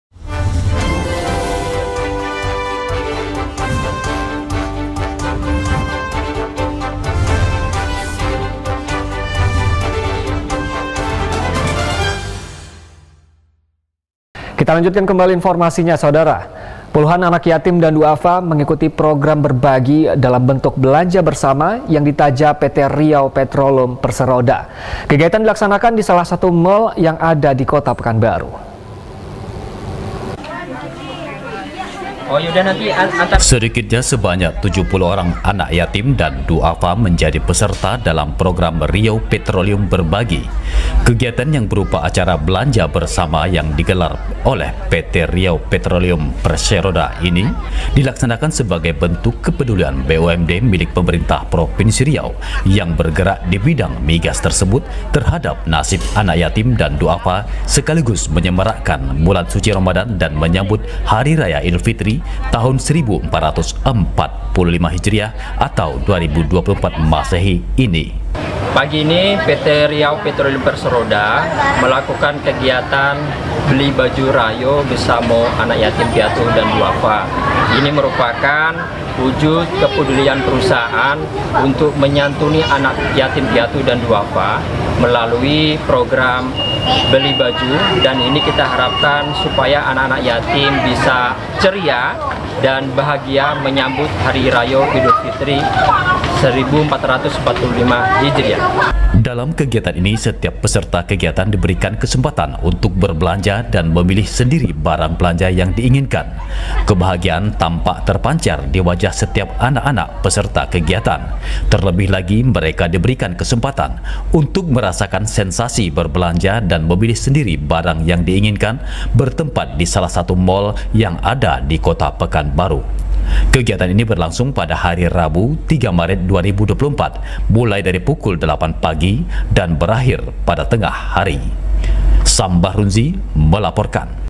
RIAU PETROLEUM BERBAGI BERSAMA ANAK YATIM DAN KAUM DHUAFA
Puluhan anak yatim dan dhuafa mengikuti program berbagi dalam bentuk belanja bersama yang ditaja pt. Riau petroleum (perseroda). Kegiatan dilaksanakan di salah satu mall yang ada di kota pekanbaru.